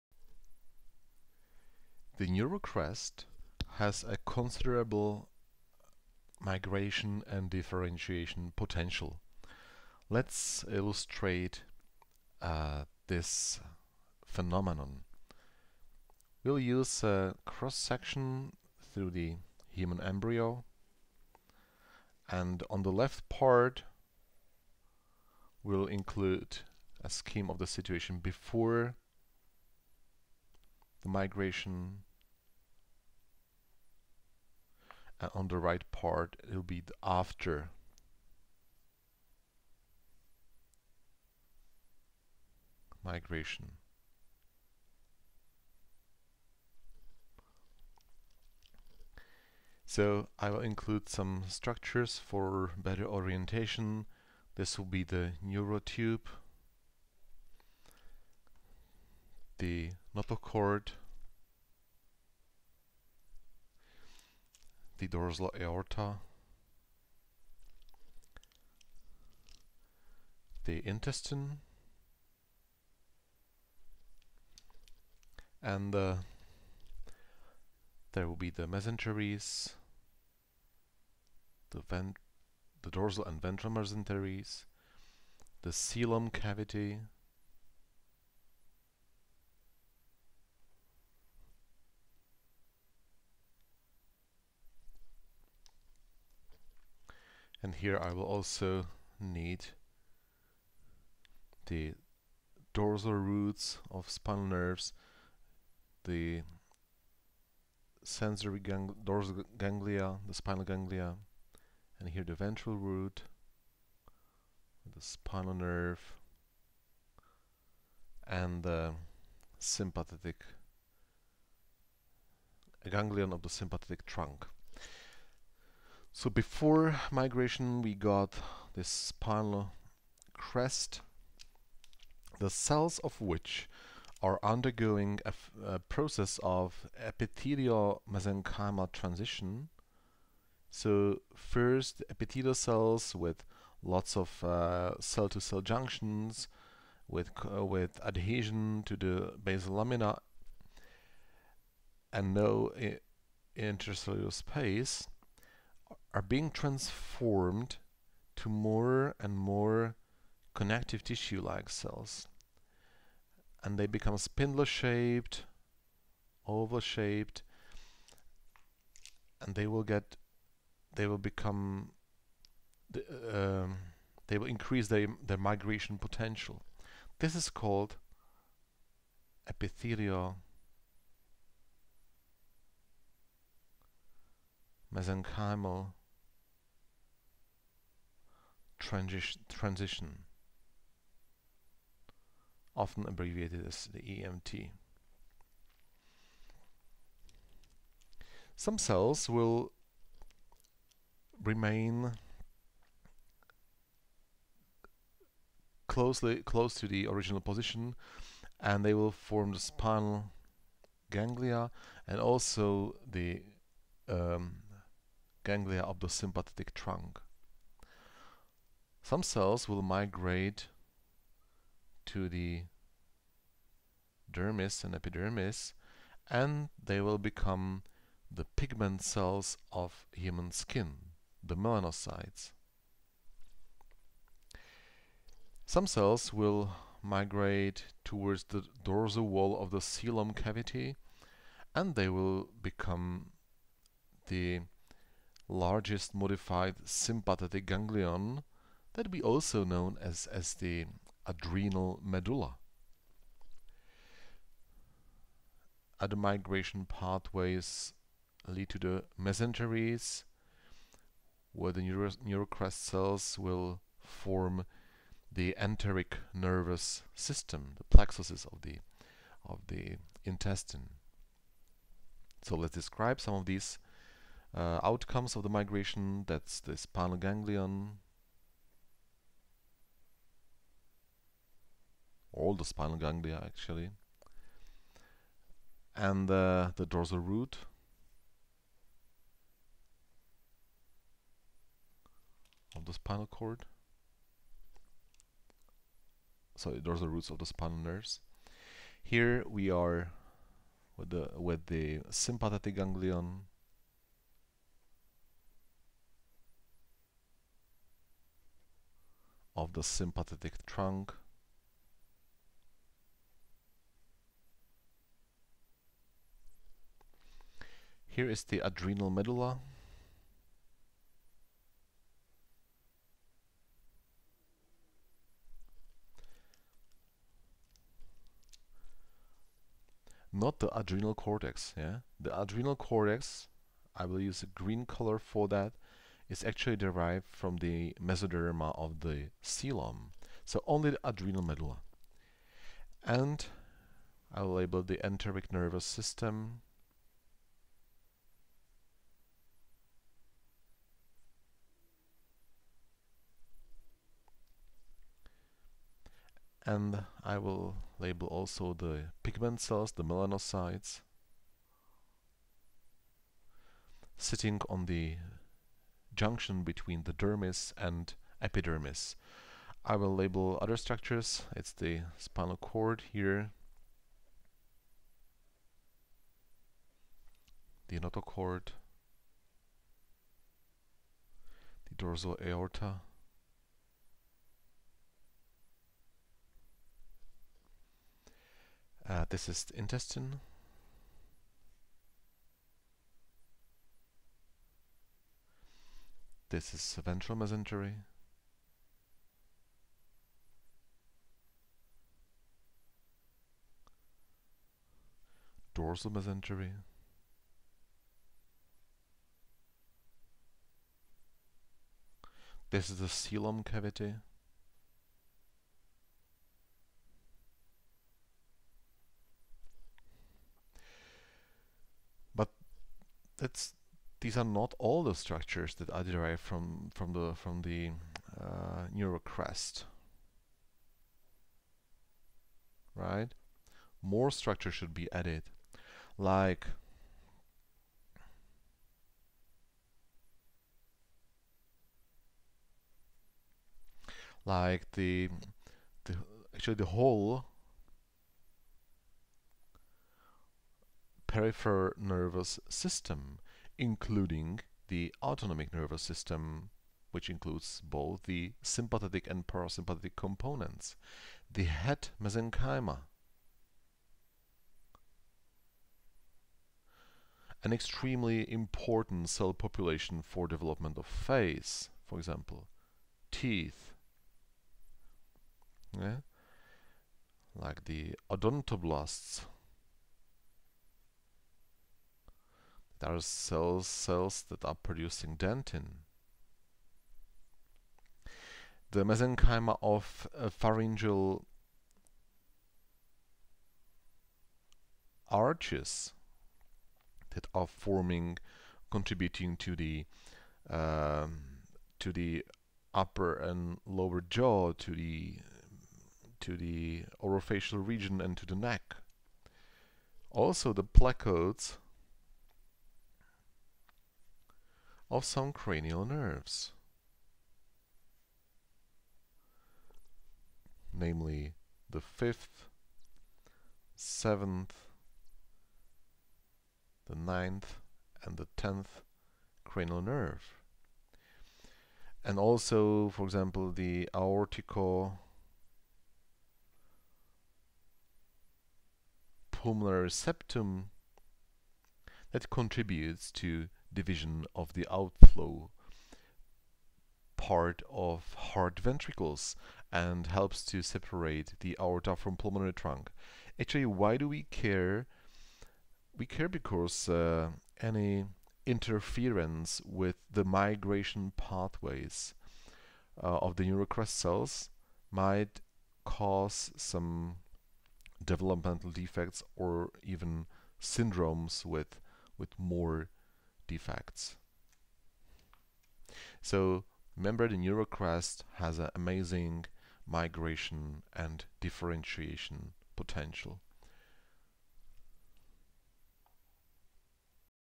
04_03 Migration and derivatives of the neural crest
Epihtelio-mesenchymal transition of neural crest cells. Their differentiation and migration through the embryonic body. Derivatives of the neural crest.

Author: Zbyněk Tonar